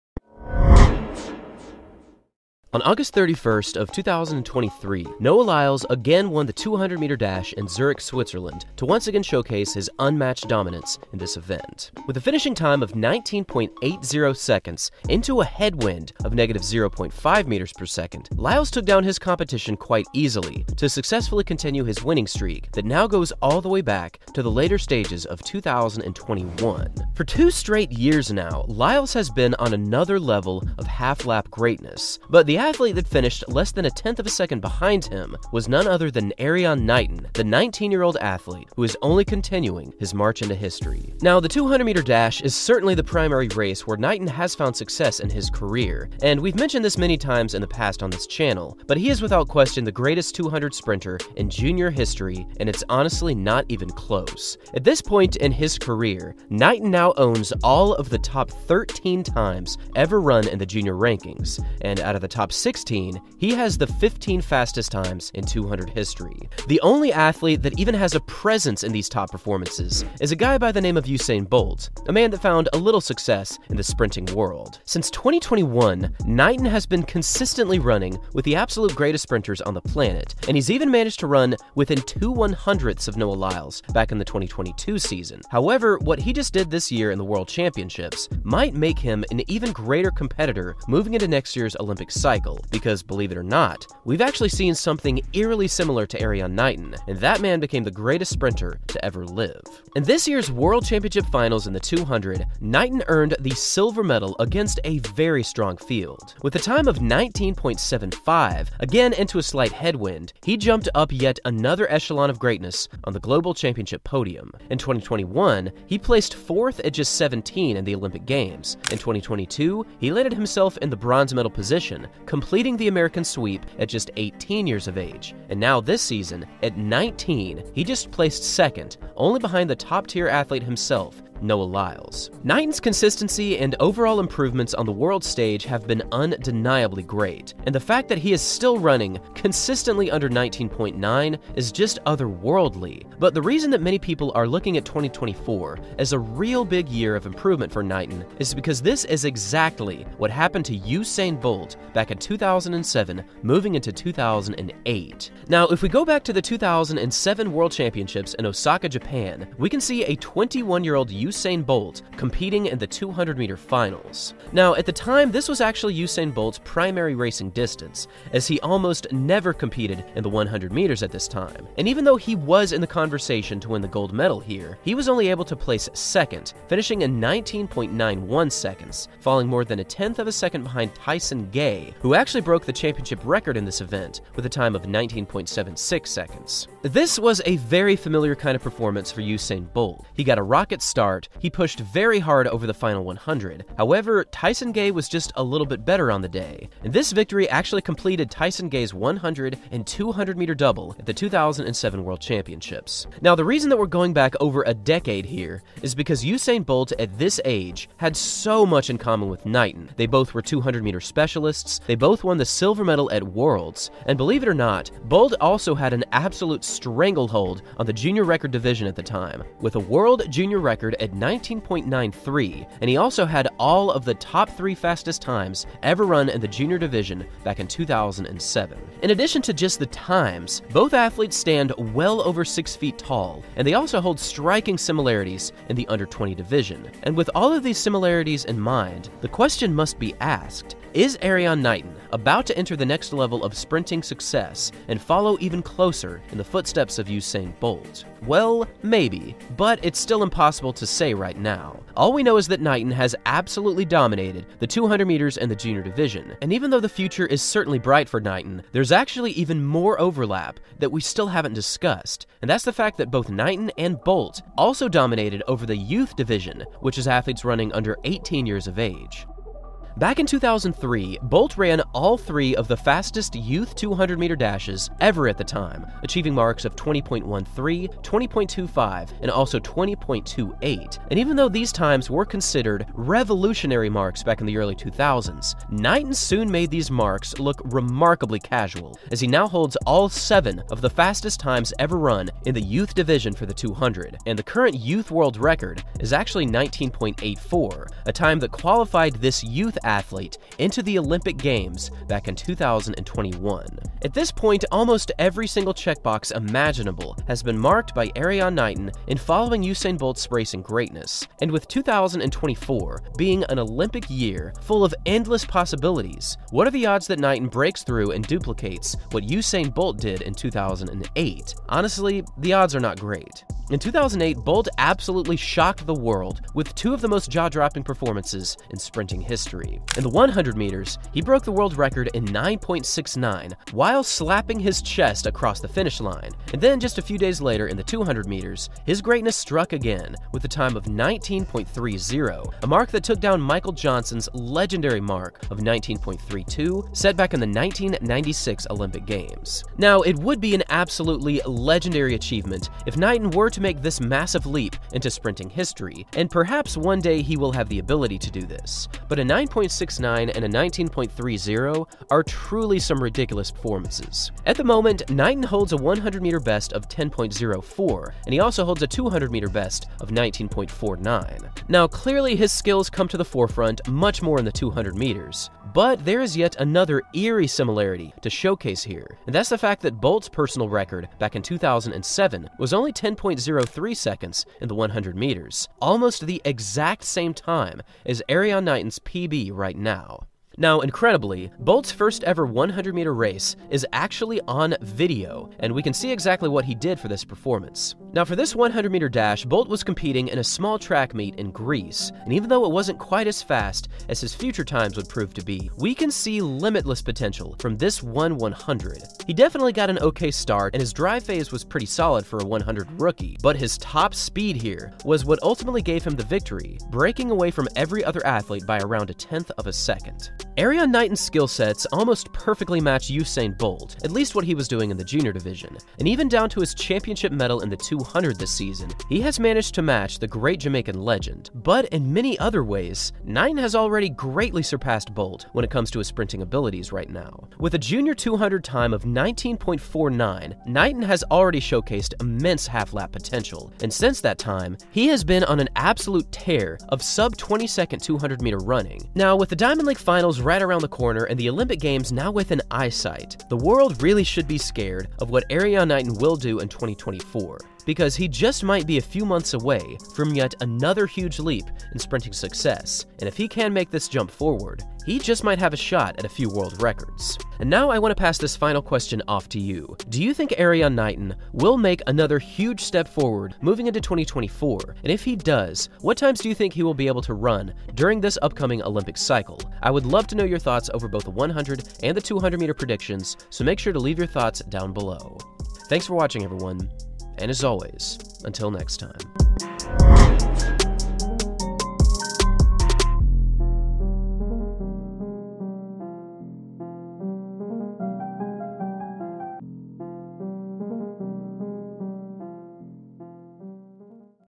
Erriyon Knighton Actually Did It
After 3 consecutive years of only getting faster, the 19 year old Knighton has officially marked his name as one of the greatest 200 athletes in the world, but will he continue his success into next year's Olympic season and perhaps make history in the 200 meters?